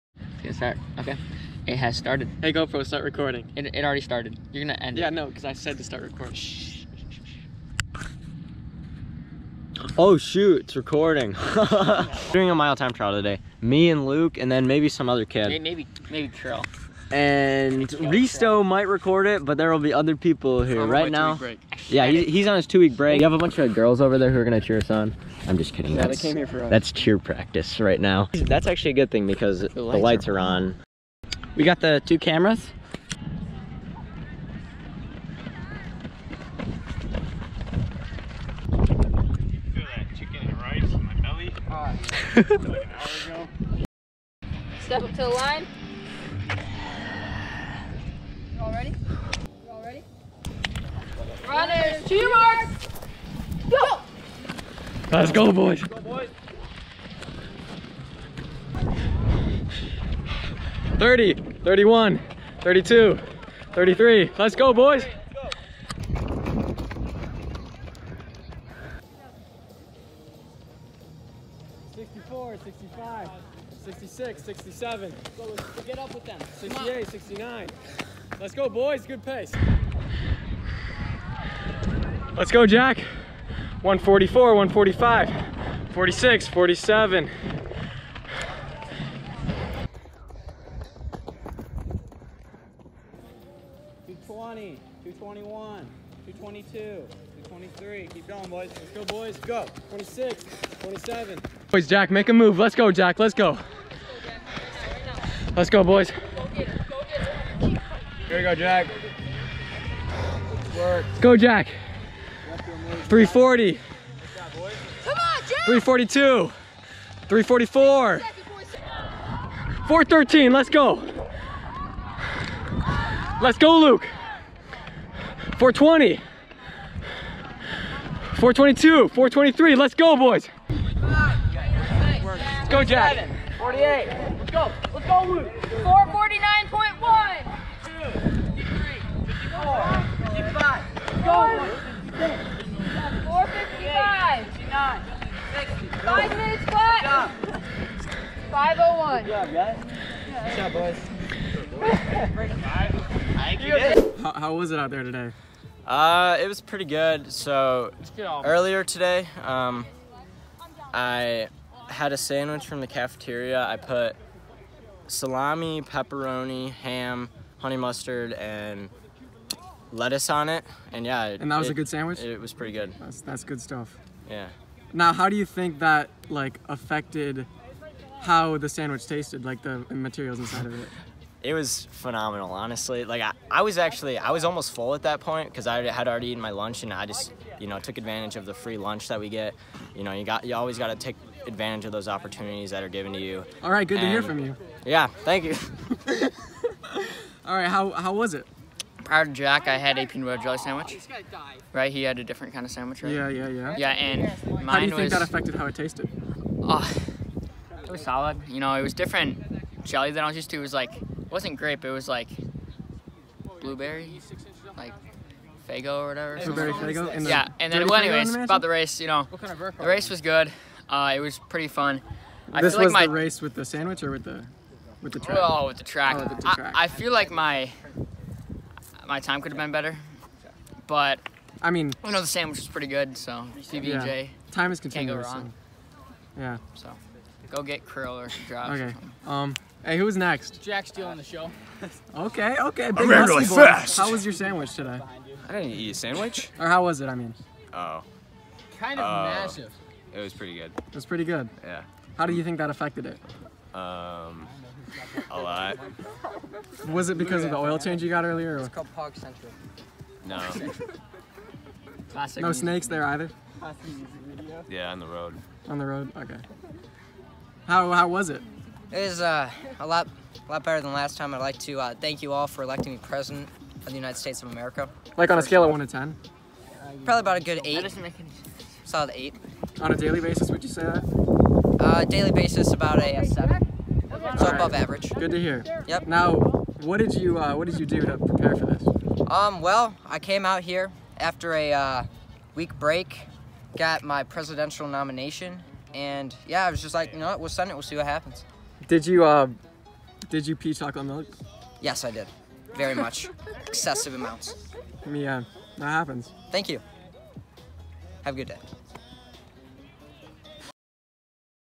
MILE TIME TRIAL UNDER THE LIGHTS
Pretty fun race. Didn’t run very well but that’ll happen. Just wanted to do that instead of a workout this week.